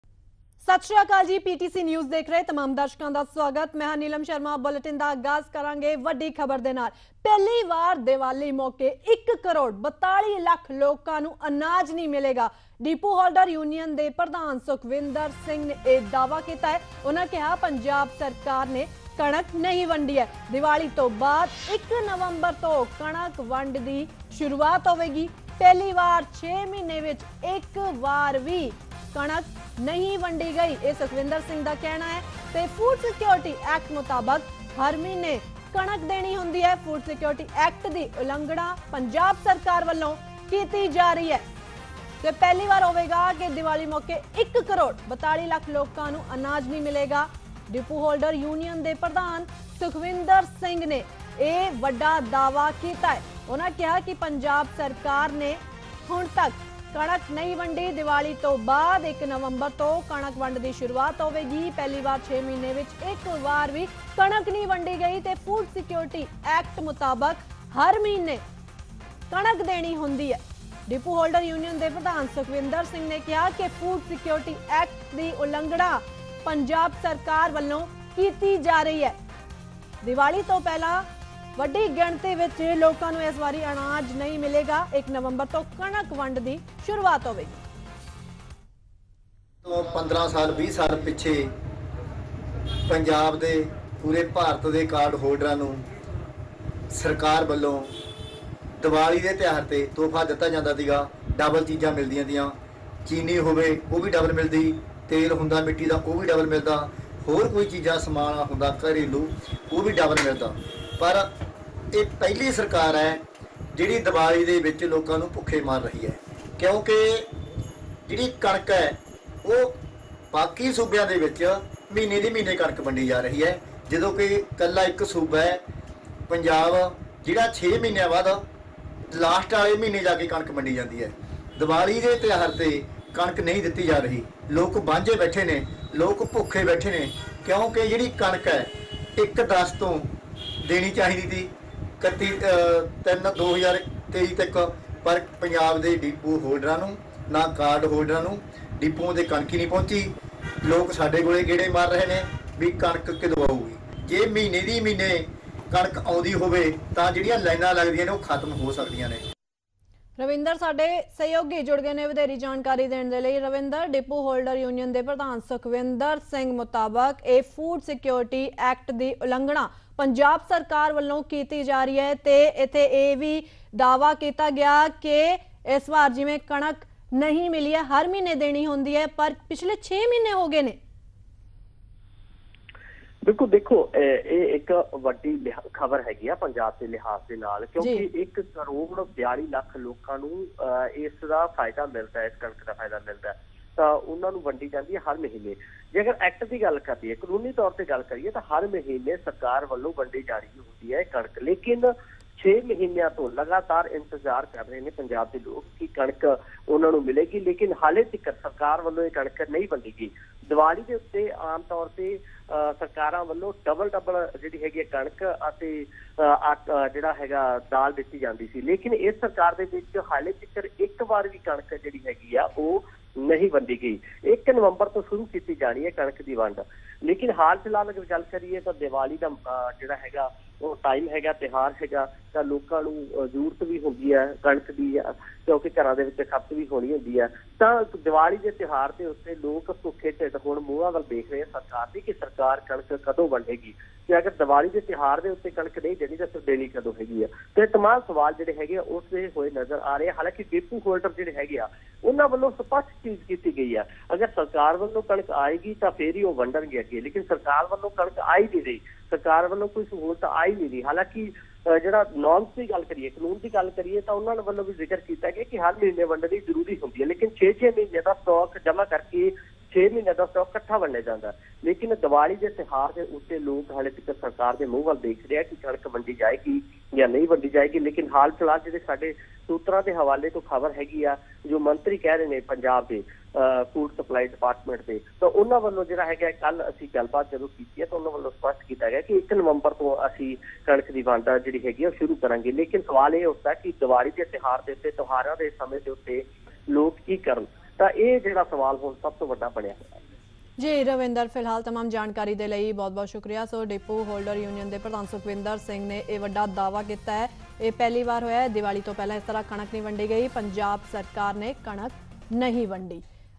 Big disclosure on distribution of wheat : ਕਣਕ ਦੀ ਵੰਡ ਨੂੰ ਲੈ ਕੇ ਵੱਡਾ ਖੁਲਾਸਾ
ਡੀਪੂ ਹੋਲਡਰ ਯੂਨੀਅਨ ਦੇ ਪ੍ਰਧਾਨ ਨੇ ਕਣਕ ਦੀ ਵੰਡ ਨੂੰ ਲੈ ਕੇ ਵੱਡਾ ਖੁਲਾਸਾ ਕੀਤਾ ਹੈ। ਡੀਪੂ ਹੋਲਡਰ ਯੂਨੀਅਨ ਦੇ ਪ੍ਰਧਾਨ ਸੁਖਵਿੰਦਰ ਸਿੰਘ ਦਾ ਕਹਿਣਾ ਹੈ ਕਿ ਪਹਿਲੀ ਵਾਰ ਦੀਵਾਲੀ ਉੱਤੇ ਇਕ ਕਰੋੜ 42 ਲੱਖ ਲੋਕਾਂ ਅਨਾਜ ਨਹੀਂ ਮਿਲੇਗਾ।